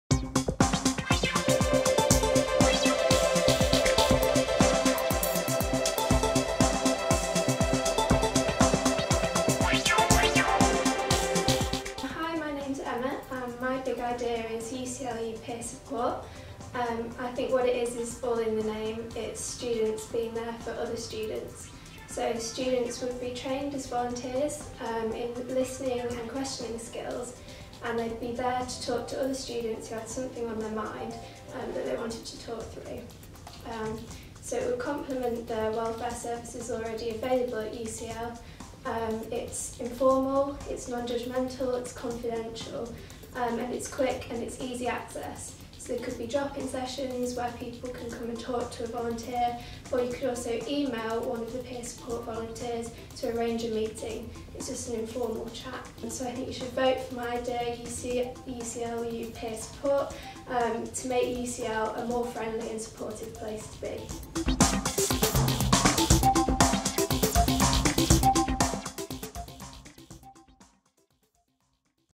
UCLU Big Ideas - Peer Support
Hi my name’s Emma and my Big Idea is UCLU peer support. I think what it is is all in the name; it’s students being there for other students. So students would be trained as volunteers in listening and questioning skills, and they would be there to talk to other students with something on their mind that they wanted to talk through.

So it would compliment the welfare services already available at UCL. It is informal, it’s non-judgemental, it’s confidential, and it is quick and easy access, so it could be drop in sessions where people can come and talk to a volunteer or you could also email one of the peer support volunteers to arrange a meeting – just an informal chat.

So I think you should vote for my idea, UCLU peer support, to make UCL a more friendly and supportive place to be.